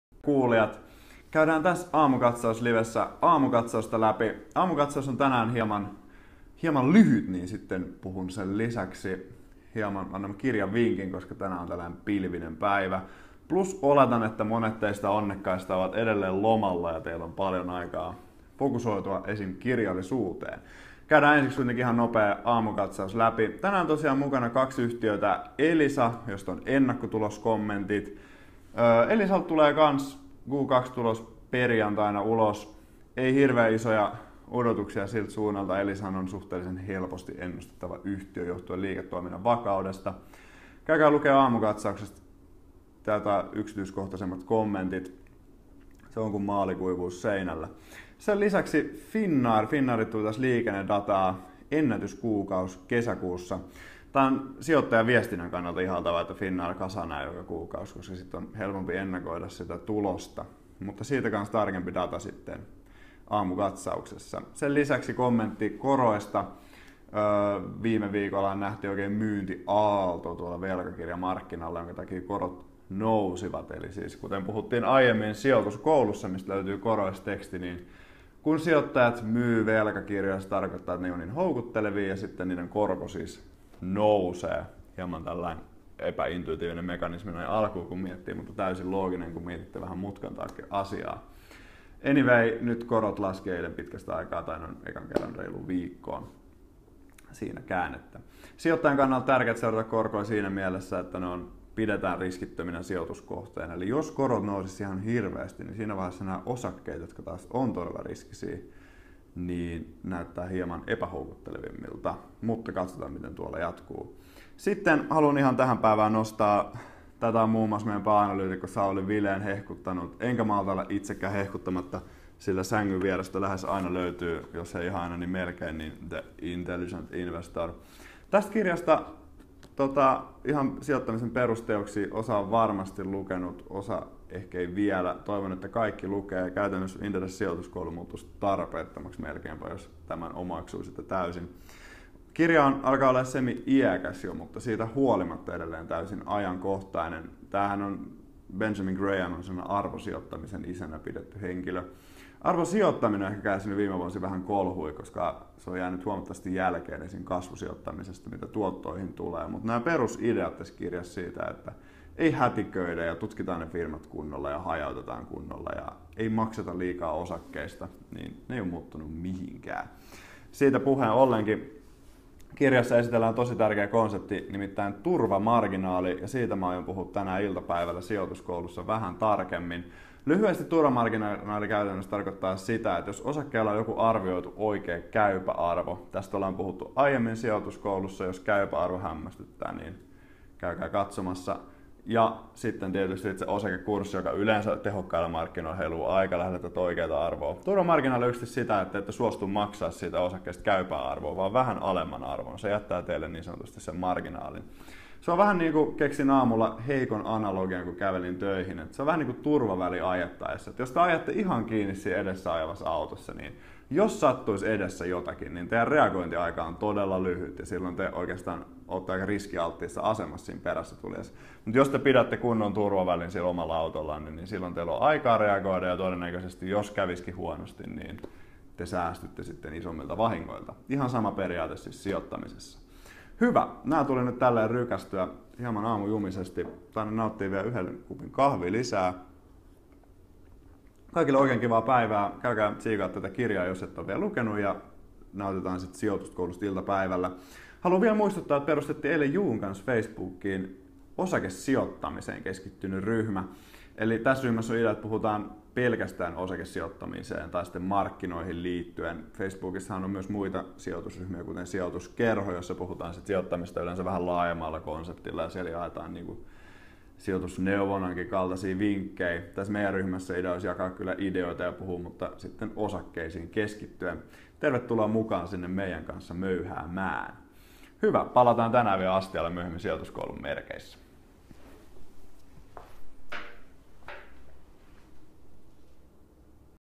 Aamukatsauslivessä Elisa ja Finnair
Aamukatsauslive:ssä puhutaan markkinoista ja Pulkkinen nostaa kirjavinkin. Mukana myös Elisan pre-tuloskommentit sekä Finnair. Lue lisää